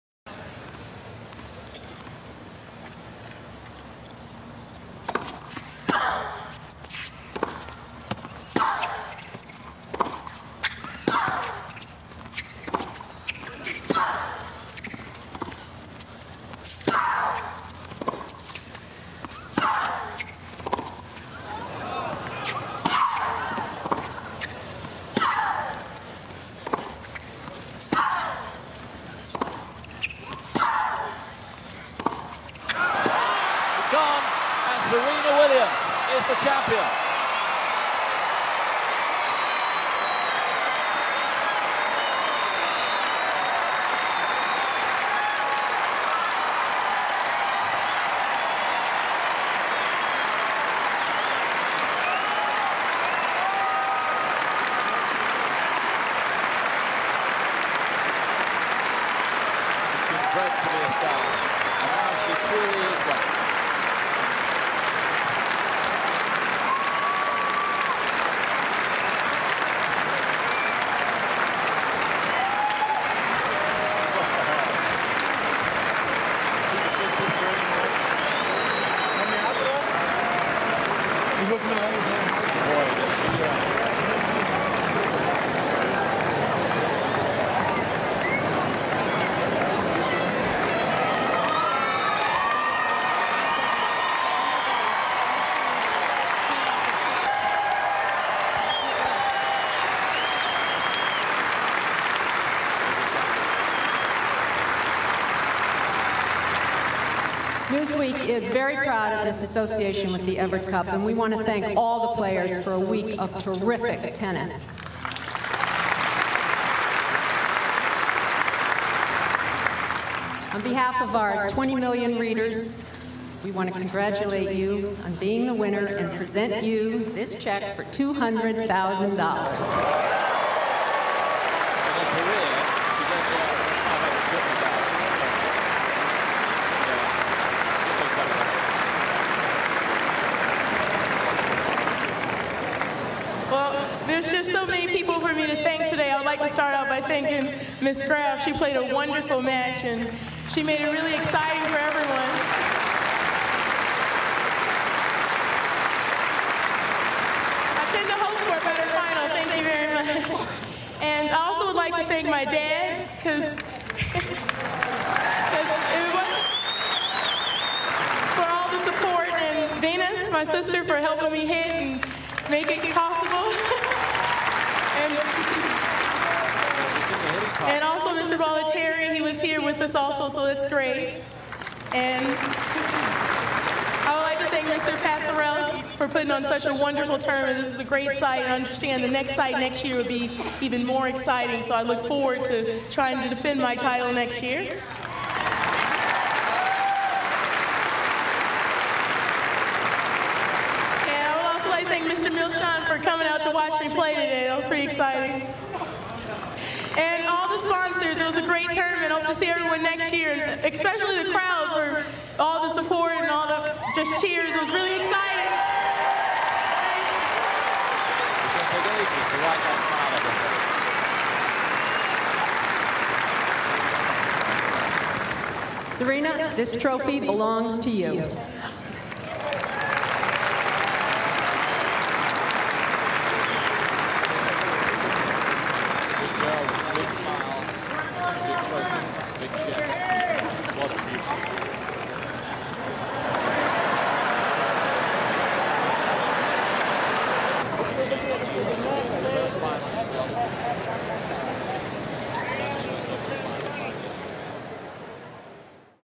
Serena Williams Defeats Steffi Graf To Win Second Career WTA Title March 13, 1999
SUPPORT THE Chavis Campbell CHANNEL HERE

FOLLOW Chavis Campbell ON ALL SOCIAL MEDIA ACCOUNTS